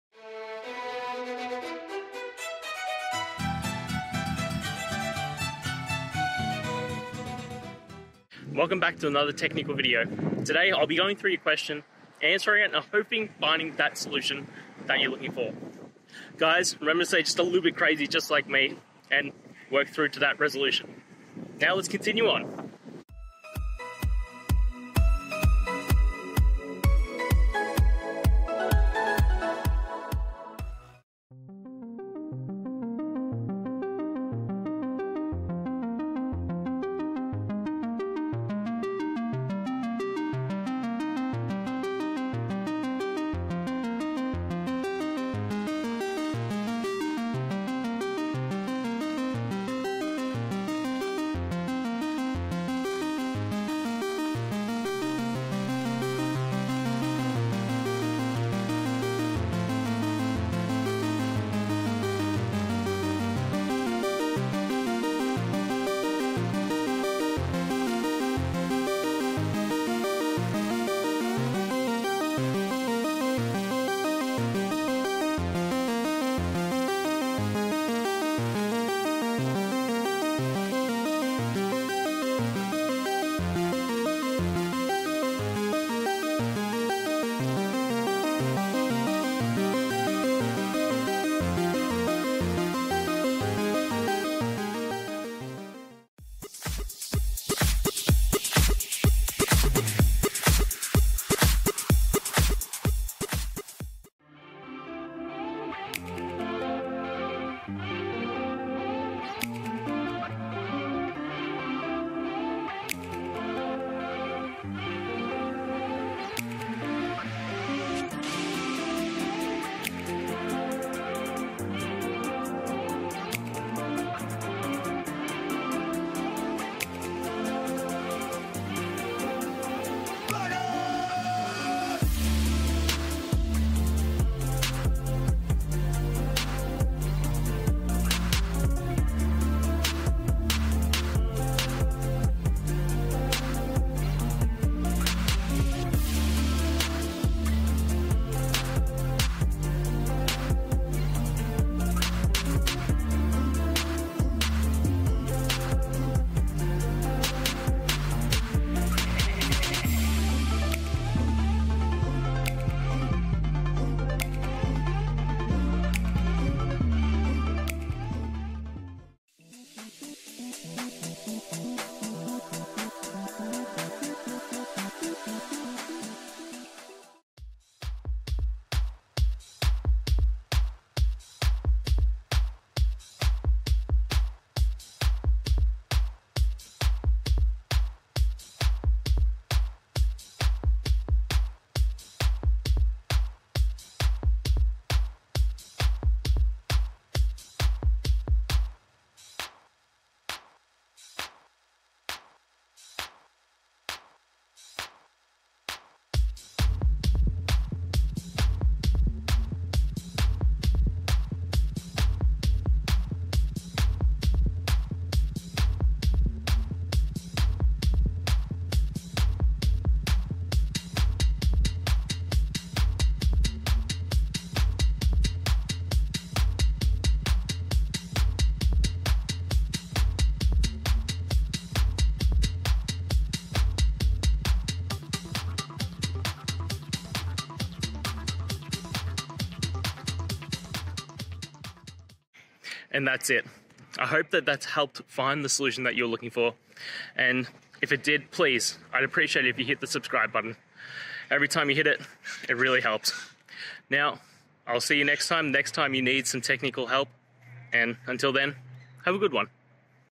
Mastering dplyr: How to Use Strings as Input in R Programming
In this video, we dive into the powerful world of dplyr, a key package in R for data manipulation. Specifically, we'll explore how to use strings as input, unlocking new ways to filter, select, and transform your data efficiently. Whether you're a beginner or looking to enhance your R programming skills, this tutorial will provide you with practical examples and tips to master string manipulation in dplyr. Join us as we simplify complex tasks and elevate your data analysis game!

Today's Topic: Mastering dplyr: How to Use Strings as Input in R Programming

Related to